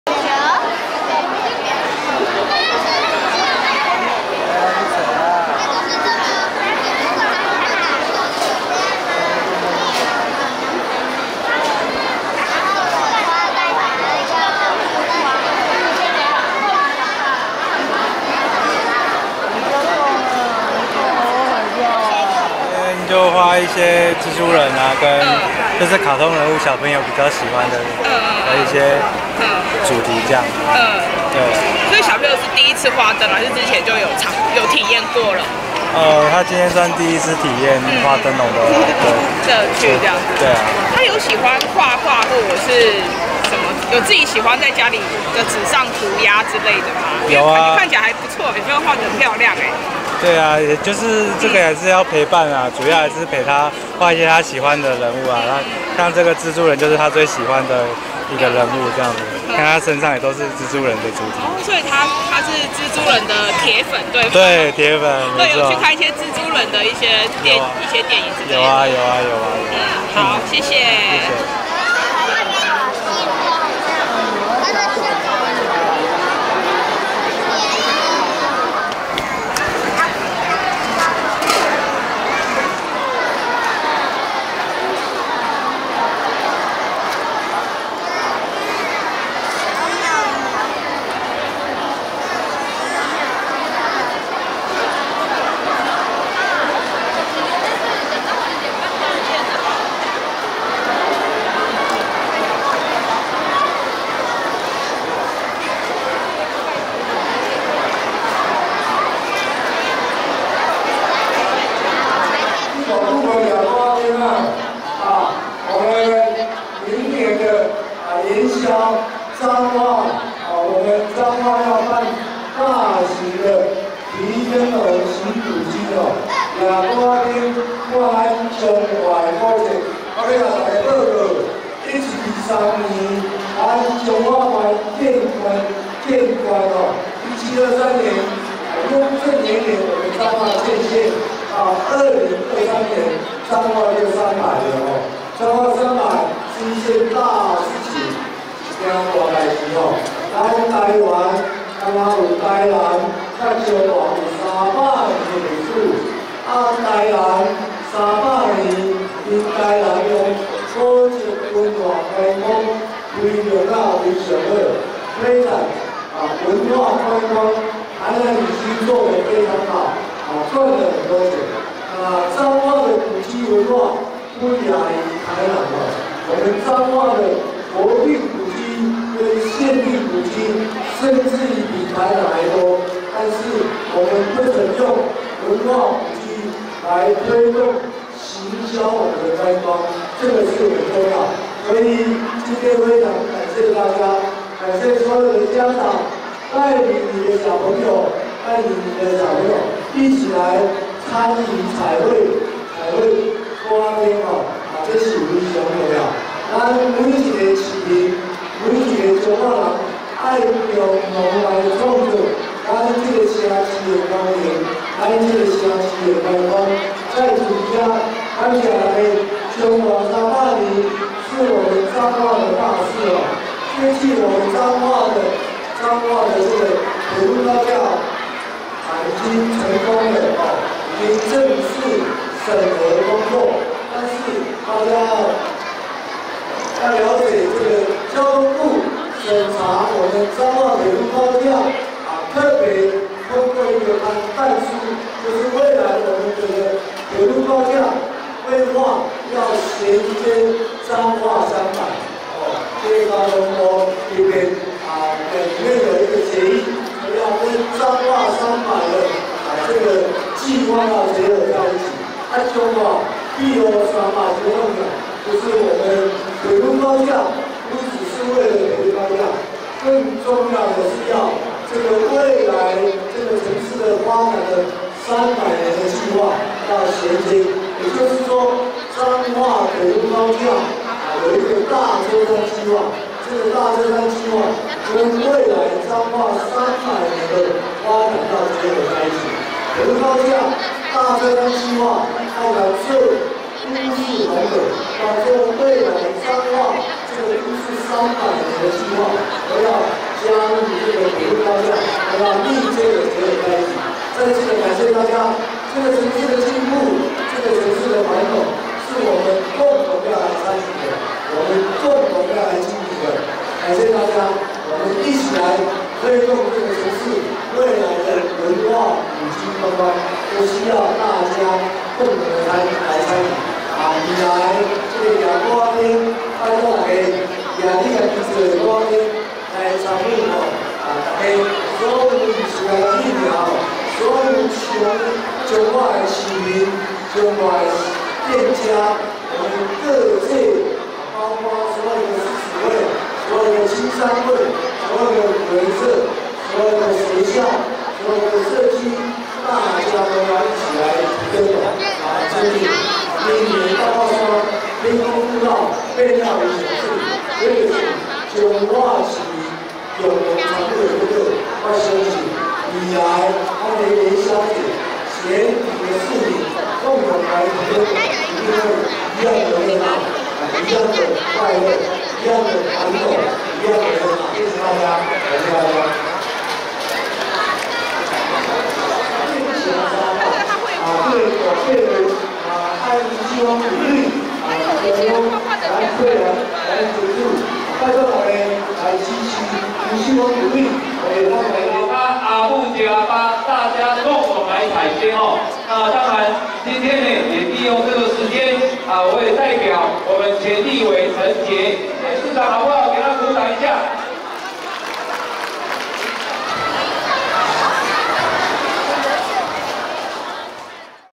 小朋友創意彩繪燈籠 為彰化建縣300年提前暖身
〔記者林明佑／彰化報導〕彰化建縣將滿三百年，縣城所在地彰化市，為迎接此一歷史性的盛事，彰化市公所積極籌劃將於明年春節期間，隆重啟動「彰化300 古城新生」系列的序幕，彰化市長林世賢邀請大家屆時一起來提燈籠踩街尋蹟，感受這片土地300年來的記憶與喜悅，共同邁入下一個300年！